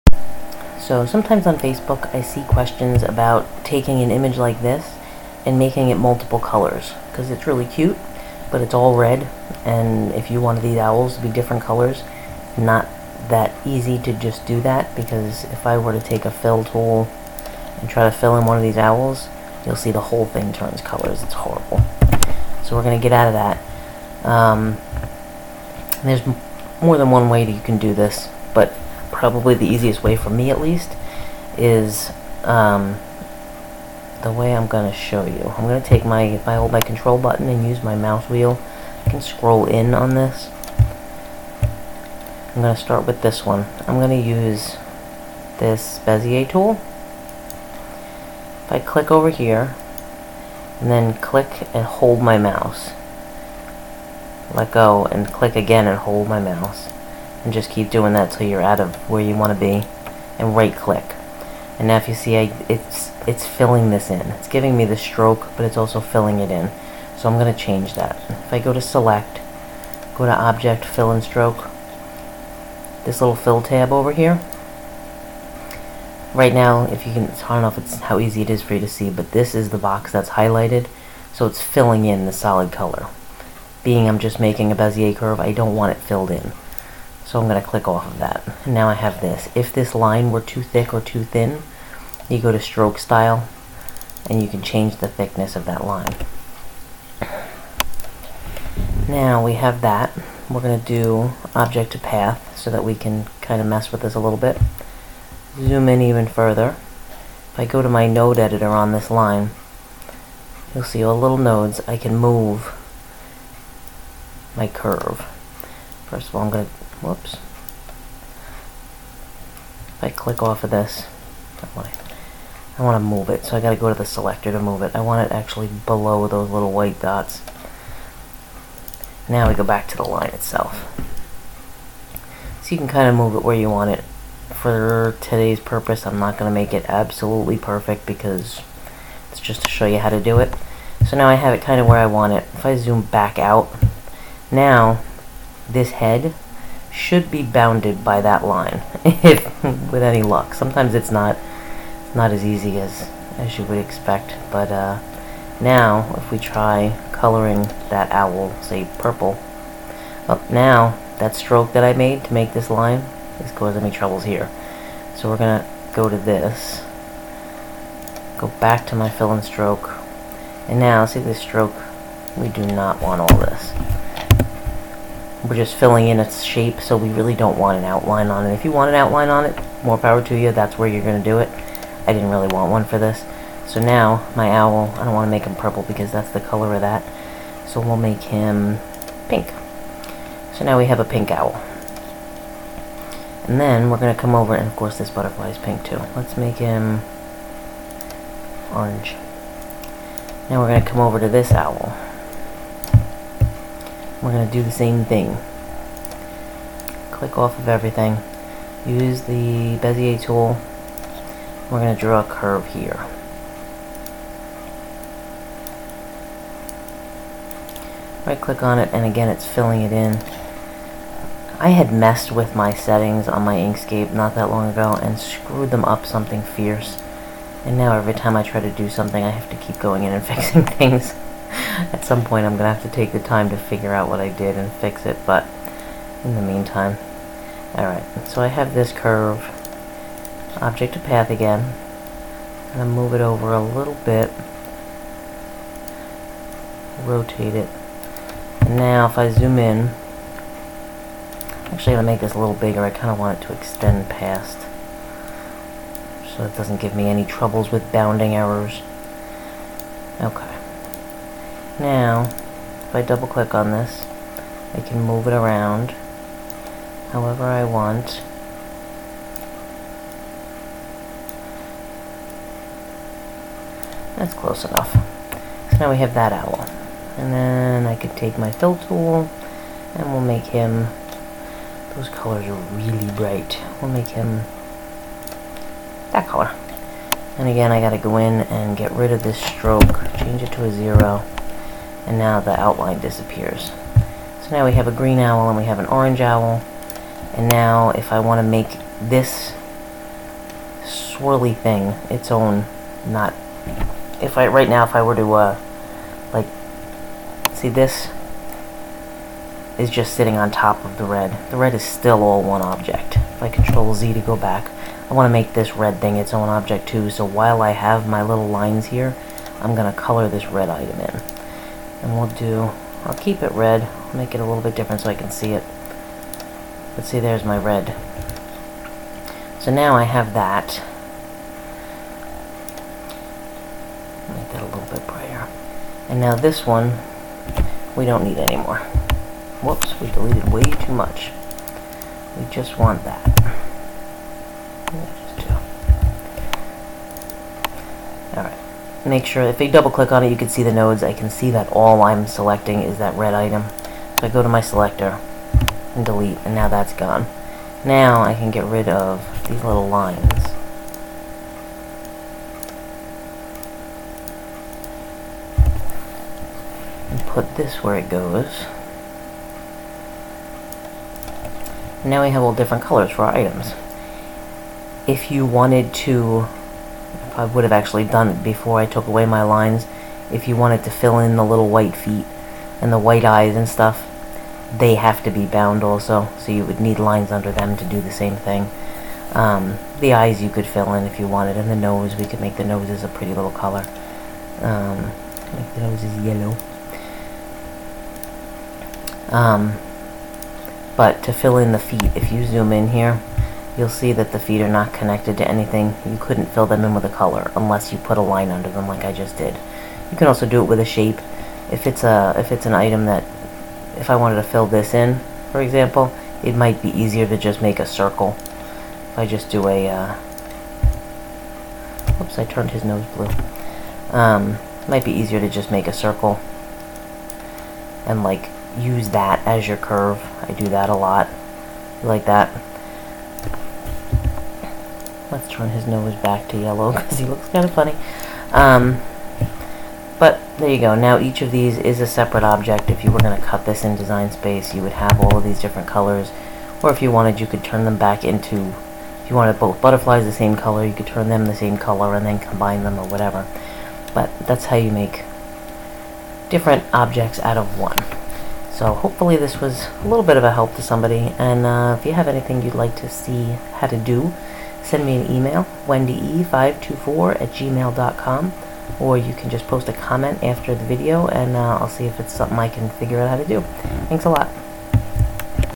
Splitting a single color design into multiple colors in Inkscape
Sometimes you have an svg (or just a regular jpg) that you love but it's all one color and you want different parts of it to be different colors. In this video I show you how I do that.